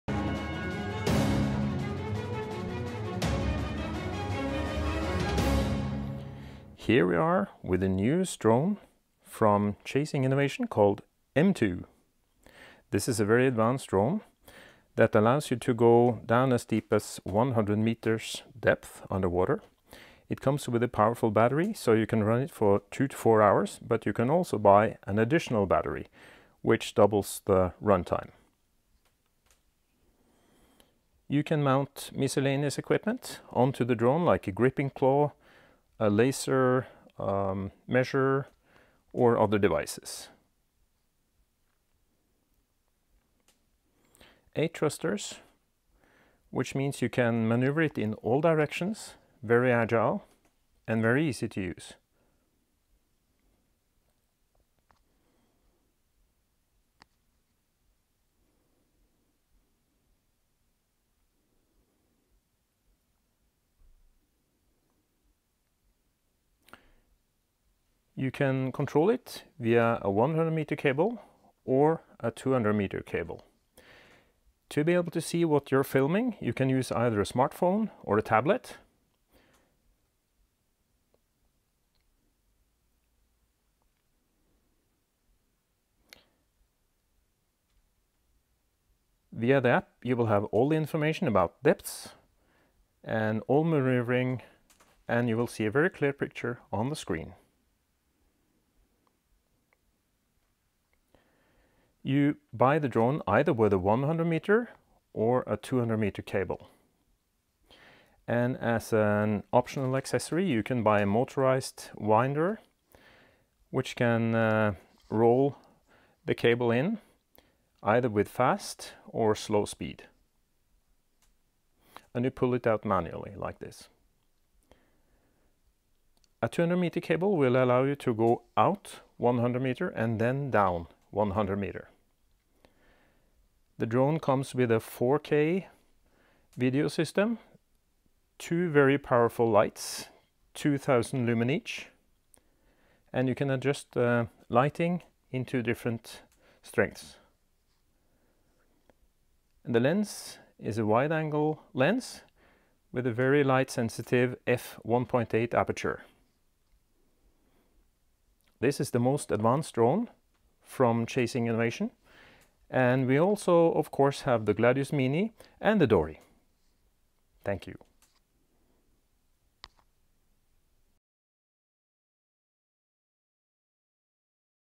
Chasing M2 - A short explanation video
In this video, Michal explain how the new Chasing M2 underwater drone works, and what features it has.